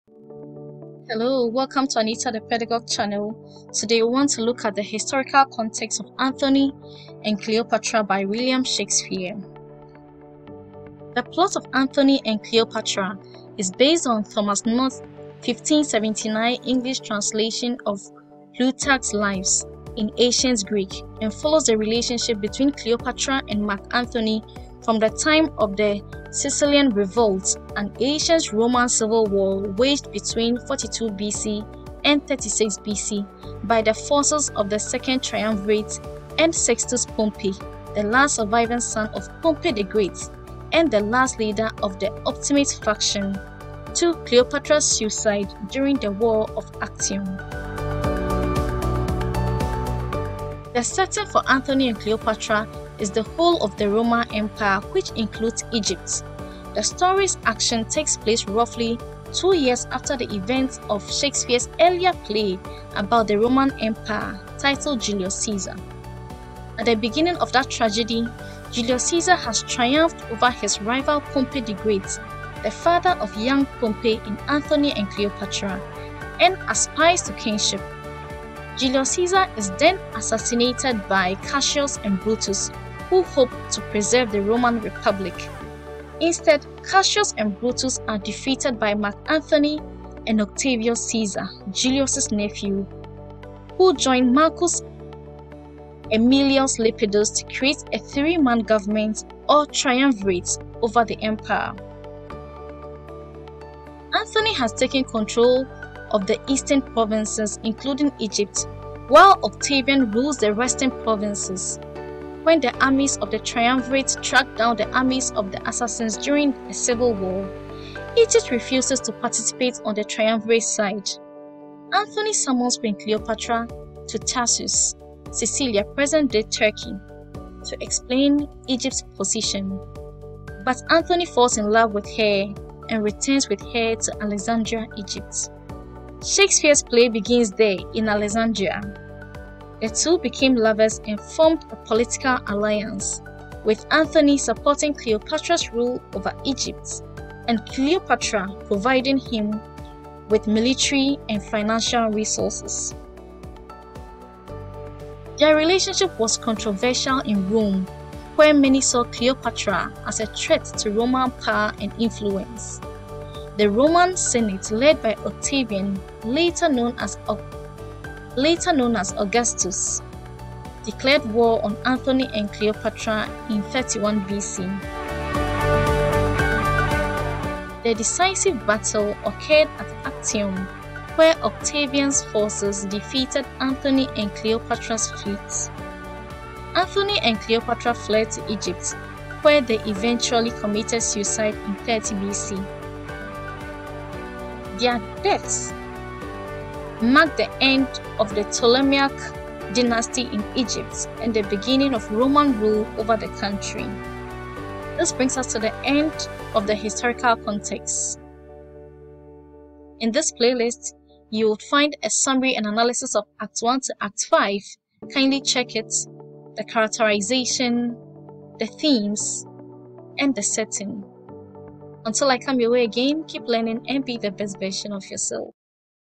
Historical Context: Anthony and Cleopatra
In this video, we'll look at where Shakespeare got the idea of Anthony and Cleopatra. You will be introduced to the historical characters that this captivating drama is woven around.
As a student, knowing this history will help you appreciate this drama better and help you prepare for examination.
You will also be given a sneak peek into the story of Julius Caesar, which Anthony and Cleopatra seem to be a continuation of.